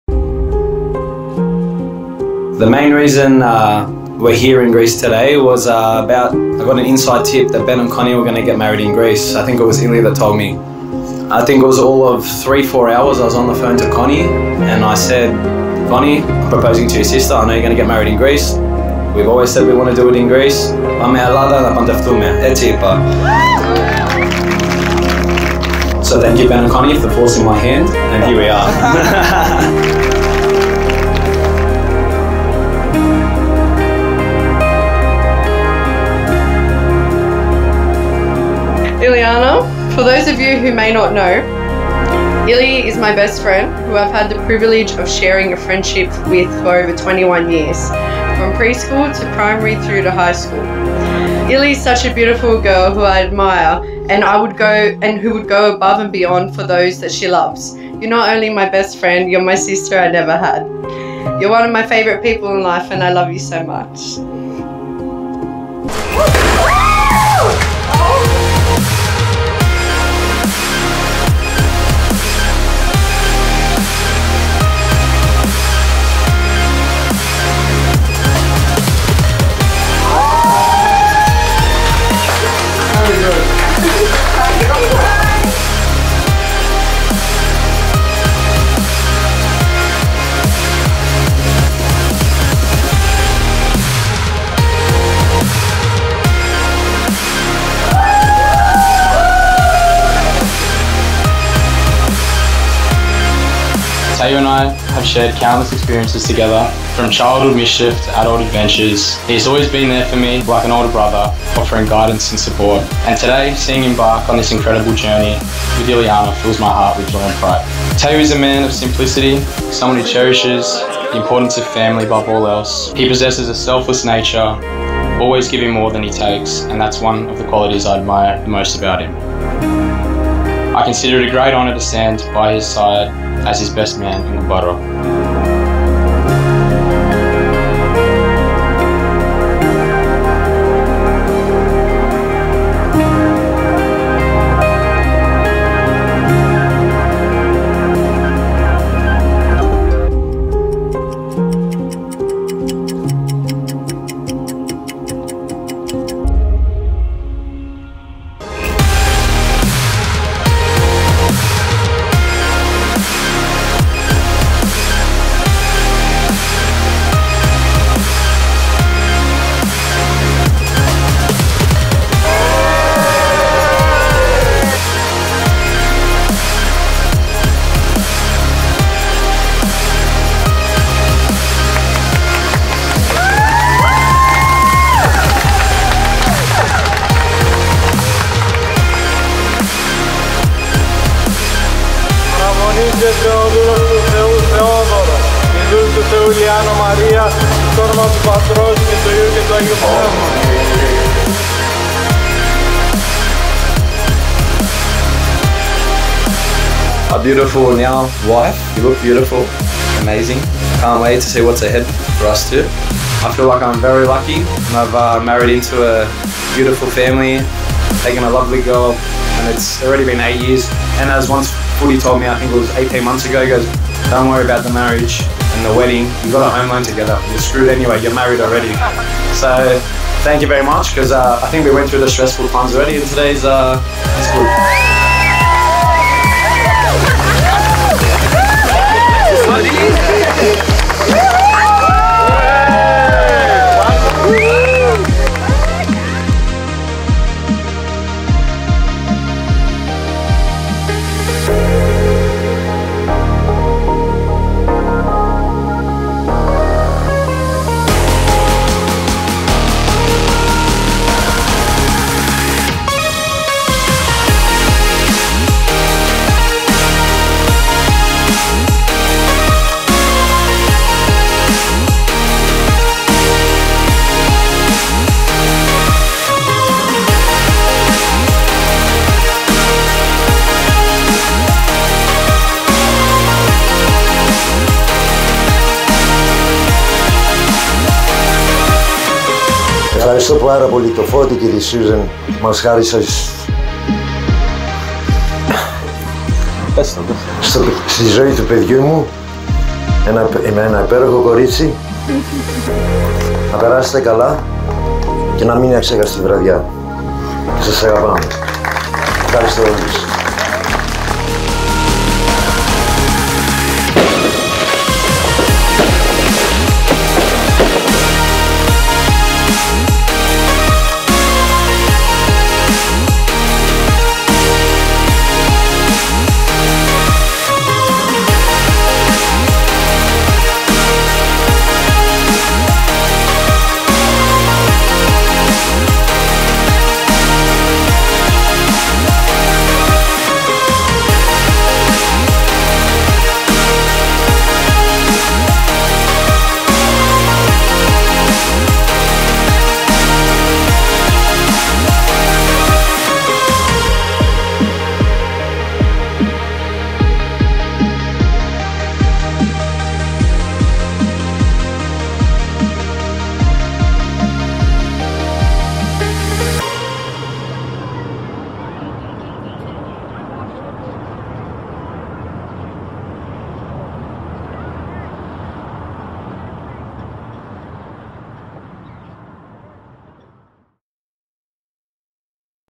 Thaao & Illiana (Destinanation Wedding Trailer)
The magical wedding of Thaao and Illiana on the enchanting Rhodes Island in Greece was nothing short of a fairy tale come to life. Set against the backdrop of the Aegean Sea, with its crystal-clear waters and breathtaking sunsets, the couple's celebration was a perfect blend of romance, tradition, and pure magic.